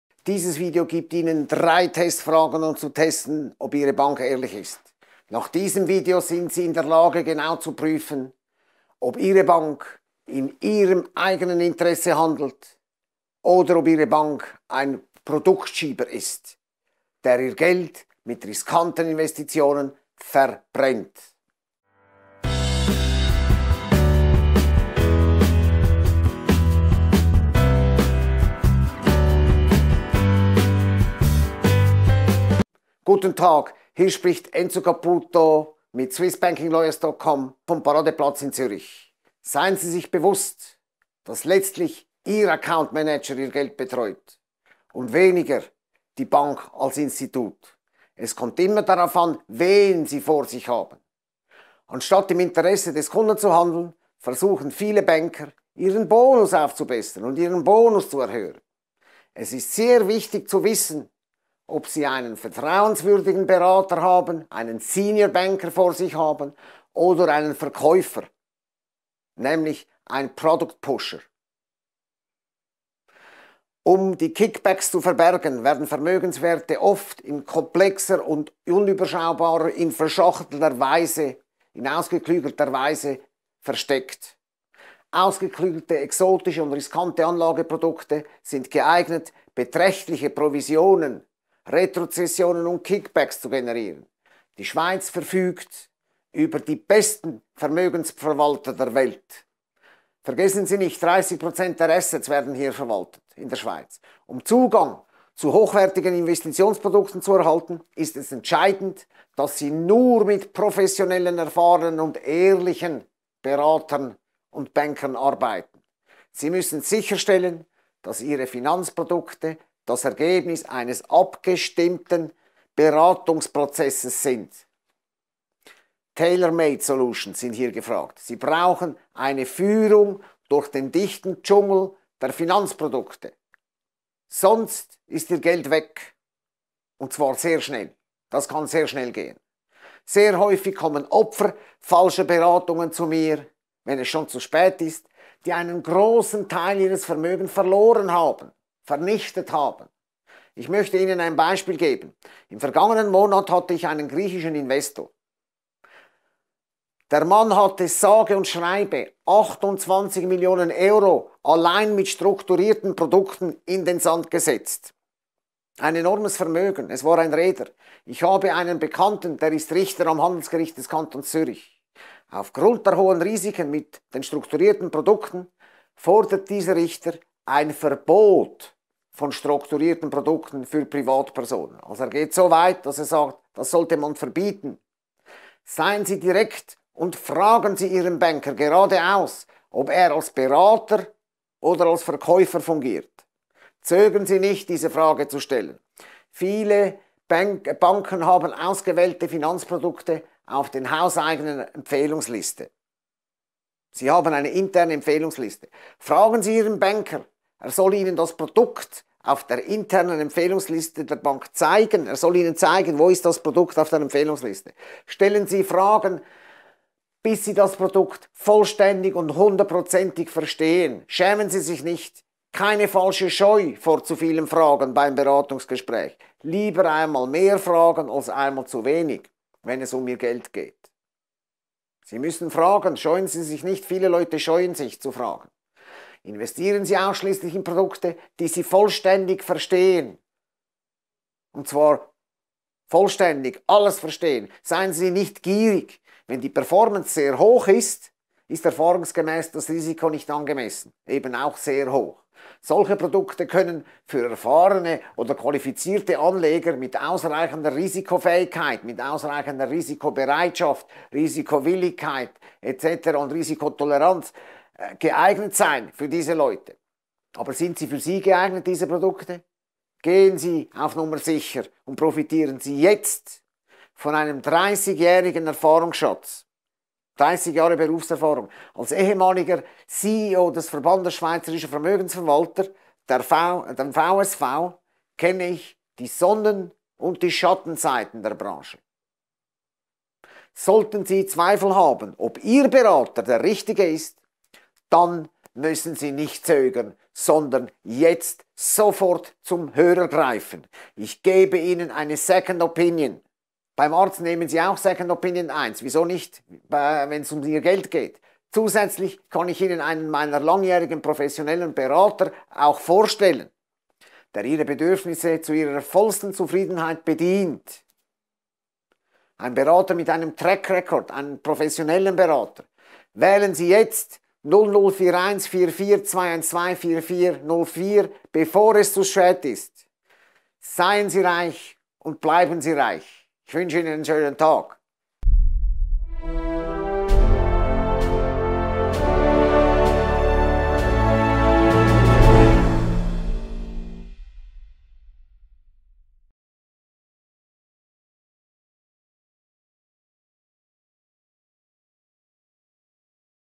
3 Testfragen ob Ihre Bank ehrlich ist
3 Testfragen ob Ihre Bank ehrlich ist

Dieses Video gibt Ihnen 3 Testfragen, um zu testen, ob Ihre Bank ehrlich ist. Nach diesem Video sind Sie in der Lage, genau zu prüfen, ob Ihre Bank in Ihrem eigenen Interesse handelt oder ob Ihre Bank ein Produktschieber ist, der Ihr Geld mit riskanten Investitionen verbrennt. ...

⇨ Multilingual service
⇨ 24/7 support